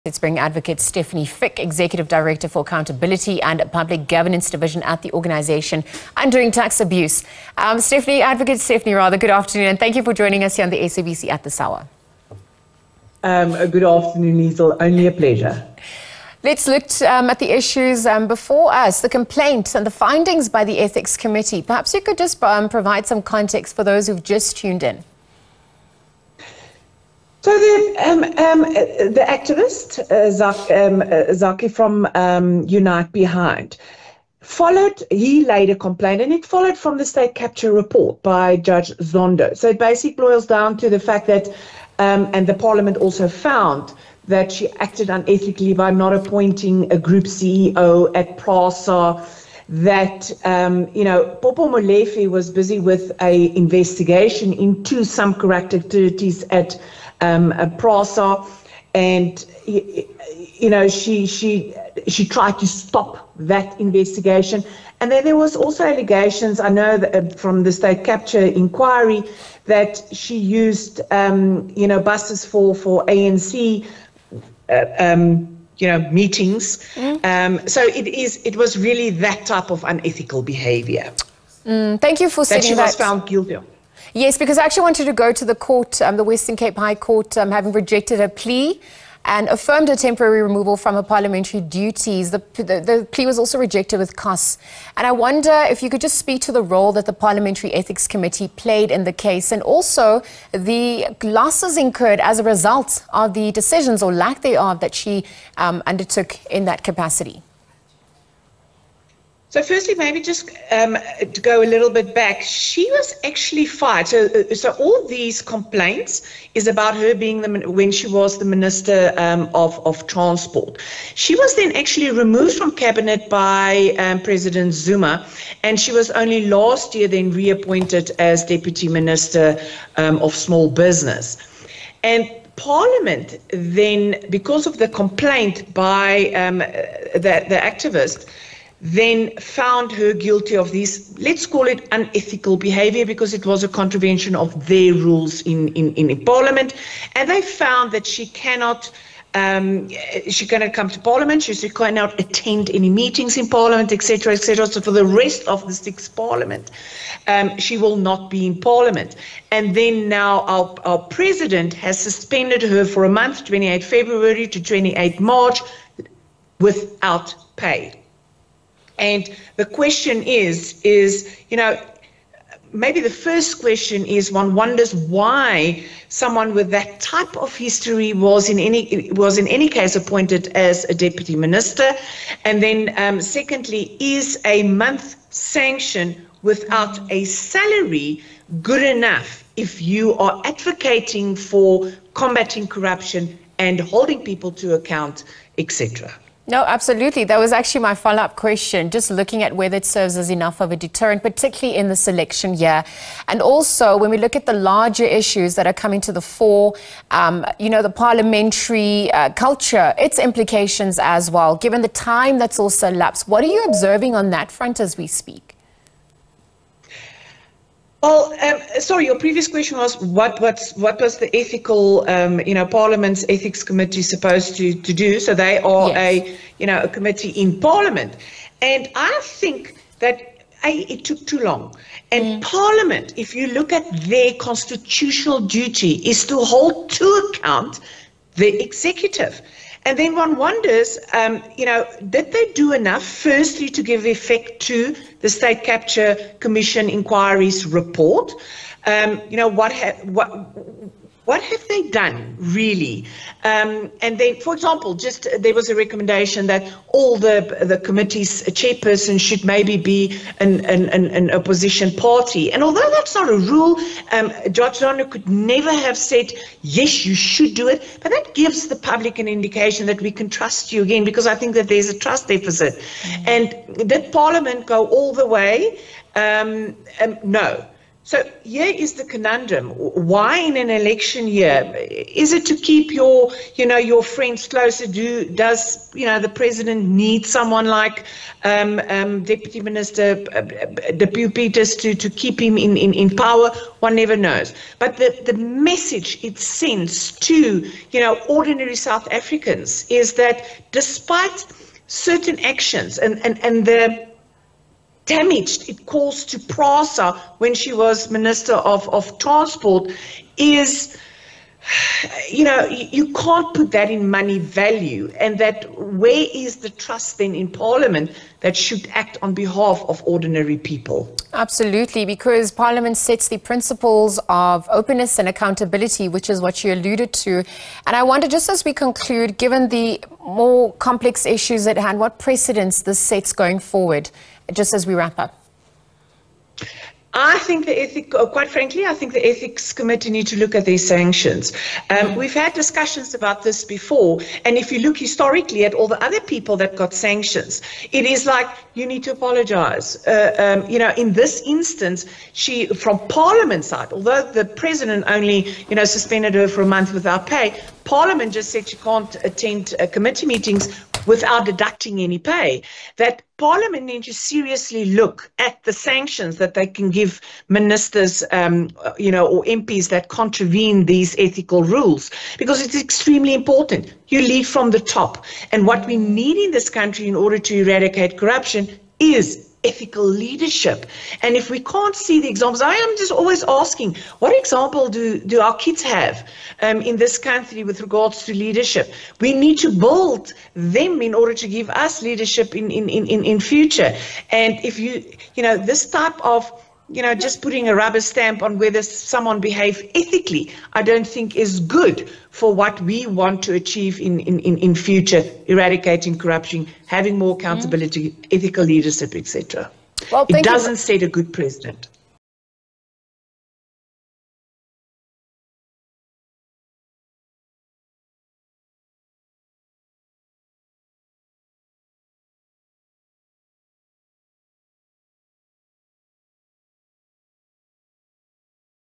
Dipuo Peters suspended without pay: Stefanie Fick weighs in
President Cyril Ramaphosa has suspended Deputy Minister of Small Business Development, Dipuo Peters, for a month. This follows a decision by Parliament's Joint Committee on Ethics and Members Interests, that she had breached the Code of Ethical Conduct in her former portfolio as Transport Minister. Let's now bring Advocate Stefanie Fick. Executive Director for Accountability and Public Governance Division at the Organisation Undoing Tax Abuse.